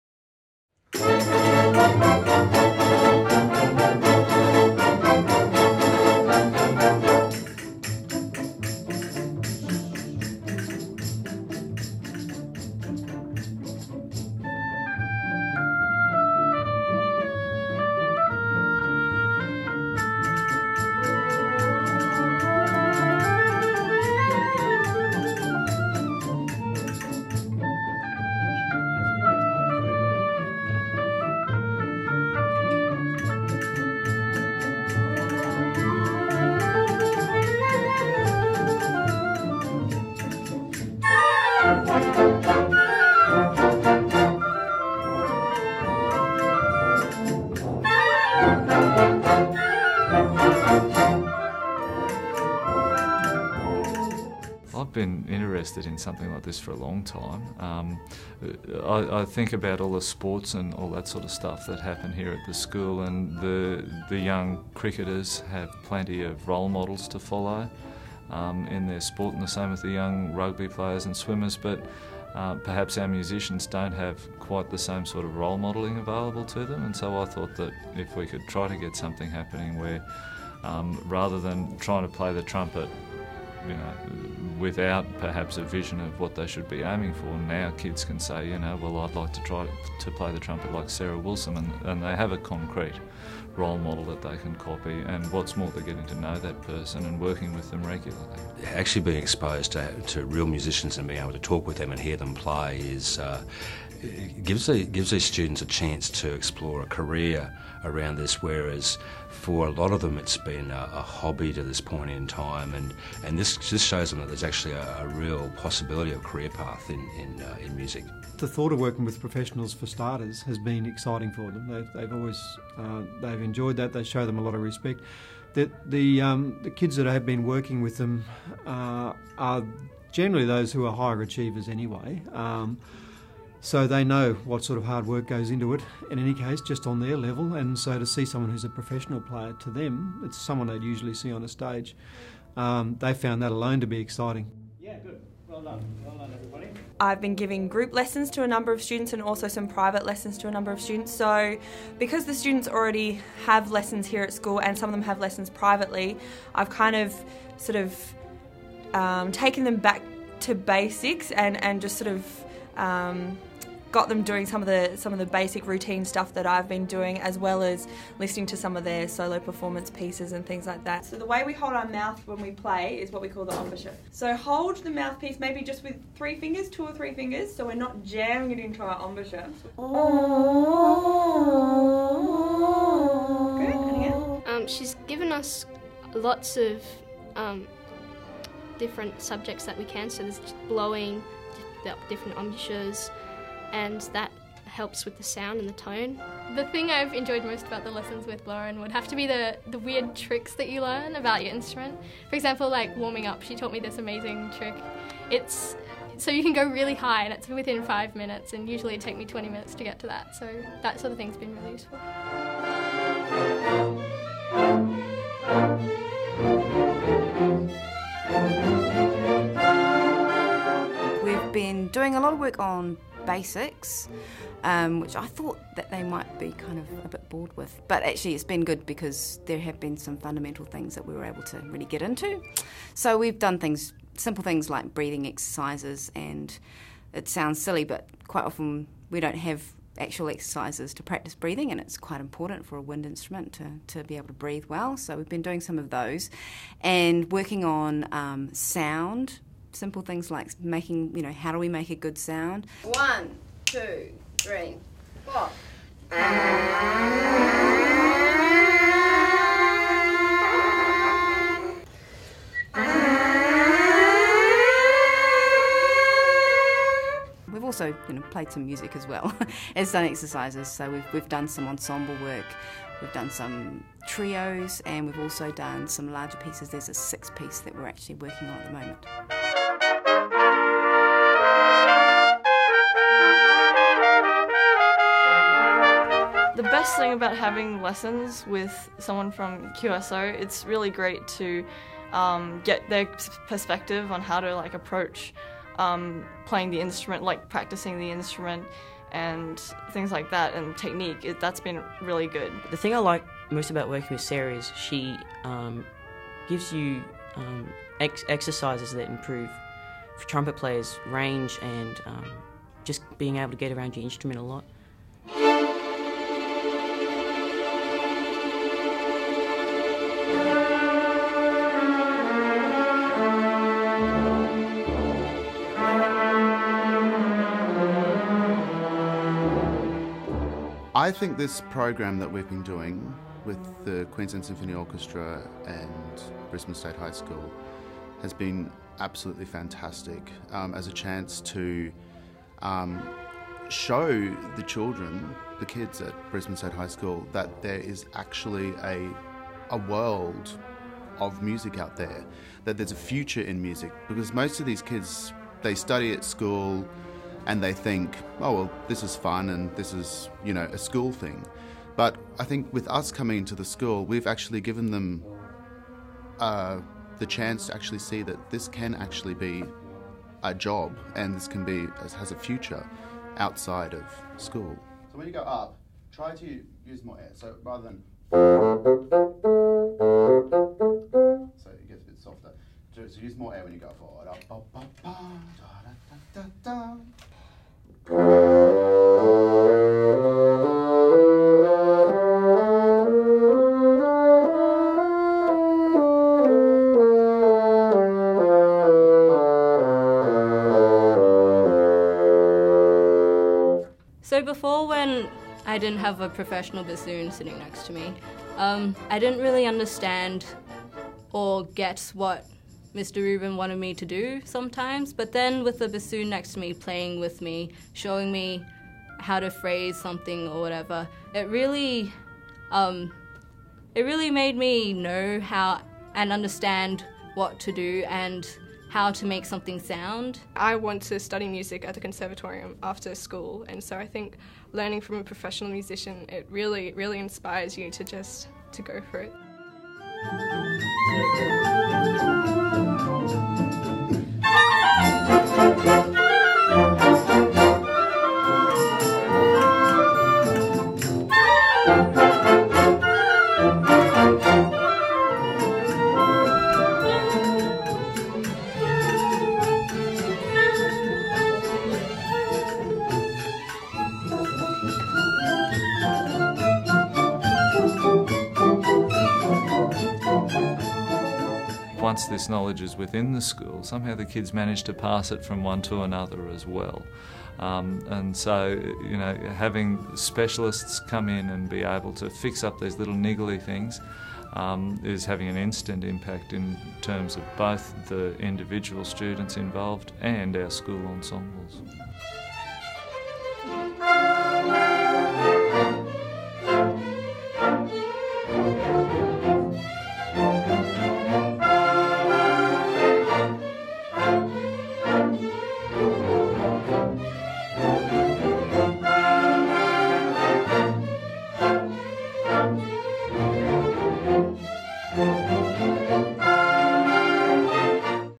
QSO Artist in Residence Project - BSHS
From term 3 2011 QSO has embarked on an Artist in Residence project at Brisbane State High School (BSHS). This partnership has a varied approach that includes specialist lessons on Trumpet (Sarah Wilson), Oboe, Bassoon (David Mitchell) and French Horn (Lauren Manuel), as well as opportunities for QSO small ensemble involvement in BSHS functions. One of the most popular aspects of this program has been the opportunity for students to attend QSO performances and hear their mentors in action on the QPAC stage.